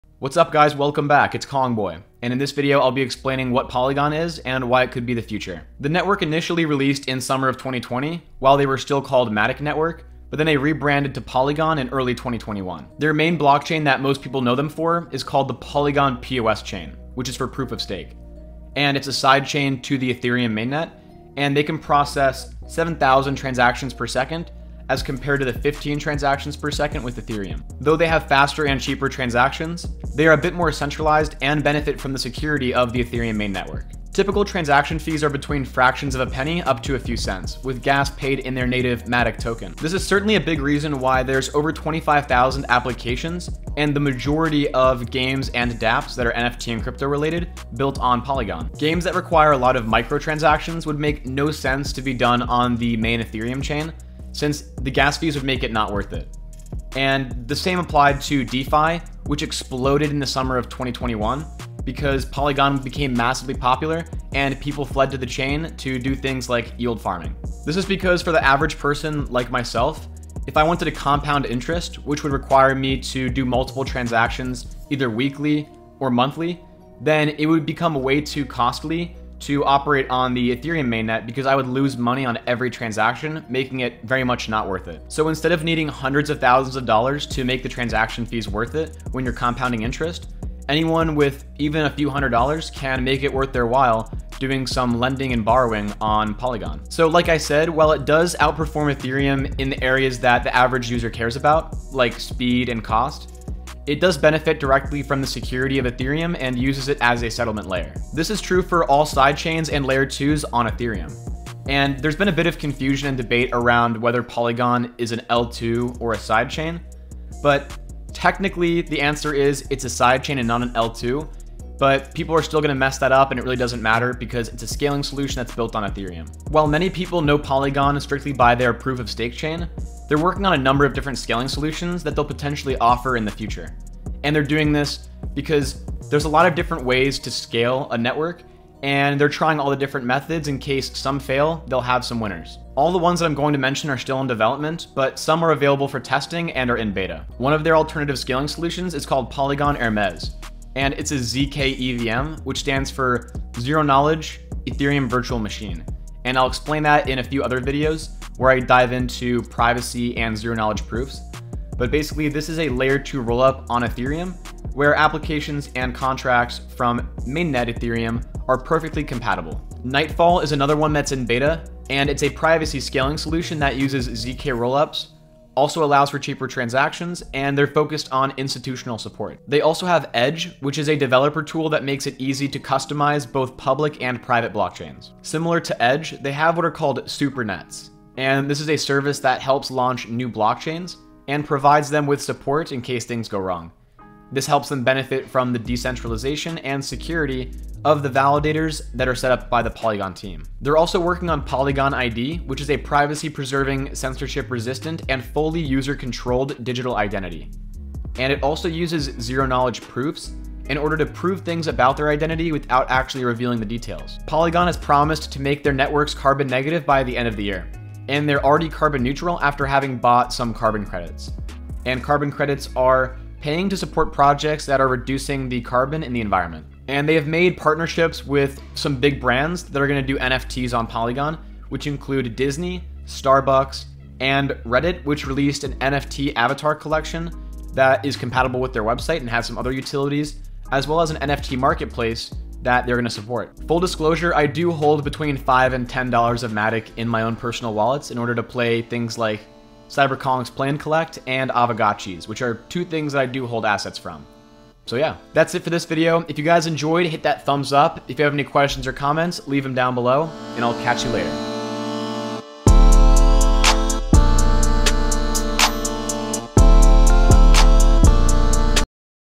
Why Polygon is the NEXT GENERATION Blockchain!
This KongBoy video explains the Polygon network scaling ecosystem built on the Ethereum blockchain. Rather than just one Polygon blockchain, there are many more blockchain scaling solutions that polygon, formerly matic network, provides. Their most popular chain is the PoS, proof of stake, Polygon sidechain to Ethereum which helps make transactions cheaper and faster. Polygon blockchains and other layer 2 solutions like roll ups benefit from the security of Ethereum and it being more decentralized. The technology behind side-chains and Ethereum L2s allow for more uses of DeFi, yield farming, and web3 gaming like blockchains games and other games on polygon. Overall, Polygon and similar networks help to solve the Ethereum scaling problem that many users face in terms of transaction fees and speed.